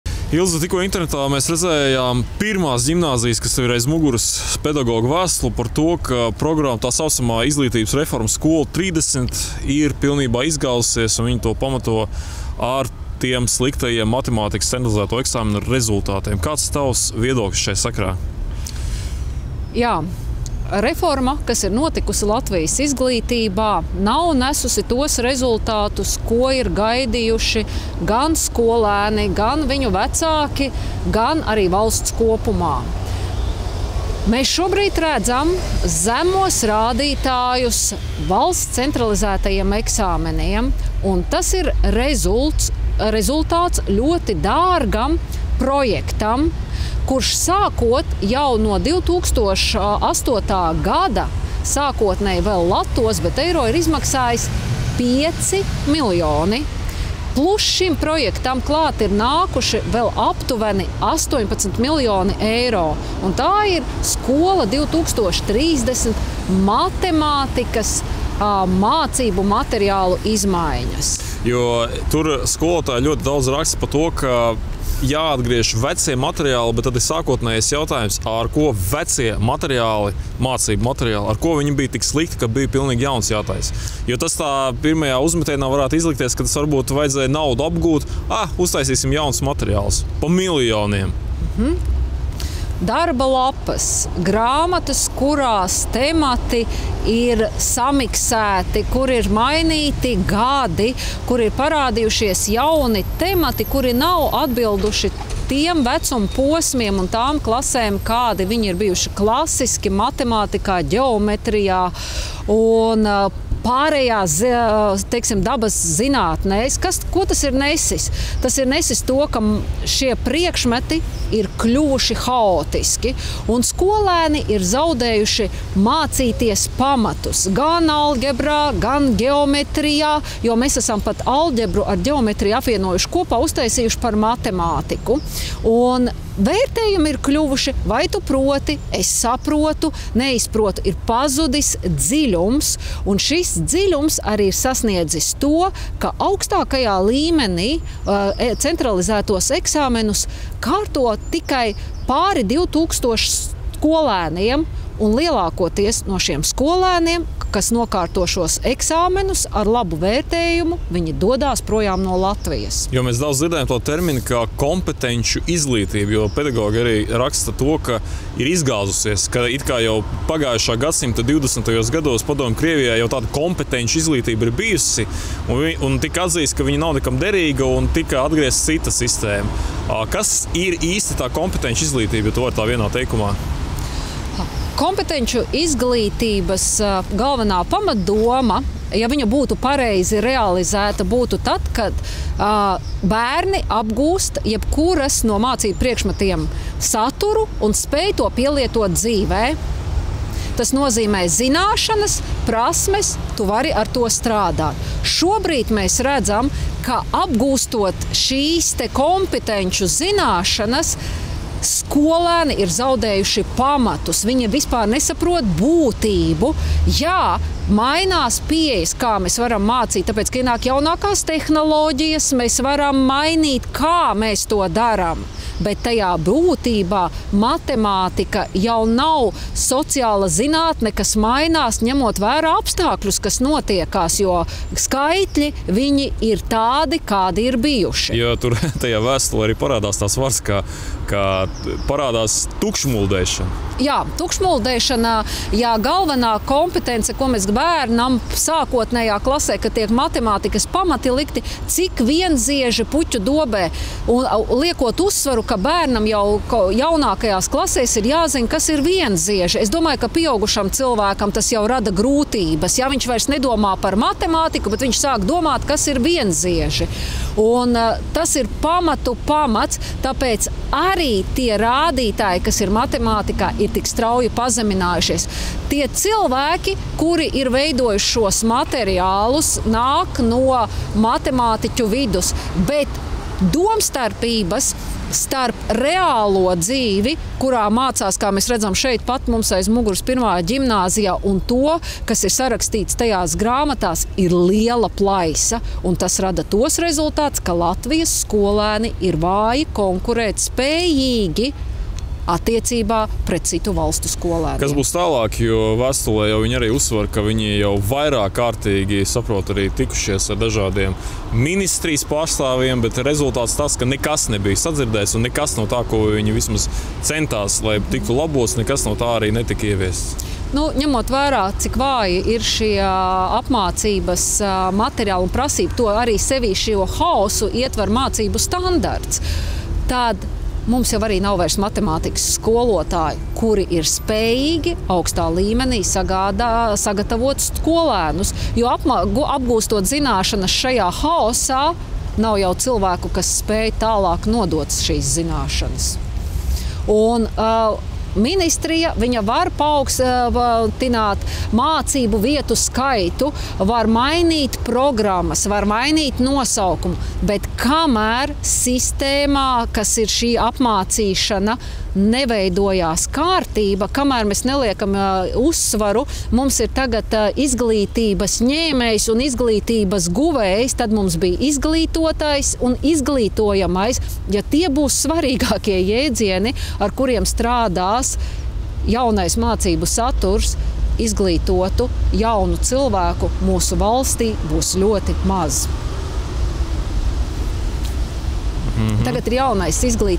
IZGLĪTĪBAS DEGRADĀCIJA LATVIJĀ - 1. ĢIMNĀZIJAS PEDAGOGU VĒSTULE VALSTS AUGSTĀKAJĀM AMATPERSONĀM
💥 Vienas no labākās Latvijas skolas - 1.ģimnāzijas, pedagogi ir nosūtījuši valsts augstākajām amatpersonām vēstuli, kurā izsaka savu viedokli par t.s. "Skola 2030" izglītības reformas sekām attiecību uz matemātikas priekšmetu❗️Secinājumi ir graujoši - šobrīd notiek Latvijas izglītības sistēmas degradācija! 📉 Rodas iespaids, ka reformas ieviešanas mērķis bija nevis uzlabot Latvijas izglītības kvalitāti, bet apgūt ES naudas līdzekļus. 💶 ILZE STOBOVA izsakās.

LATVIJA PIRMAJĀ VIETĀ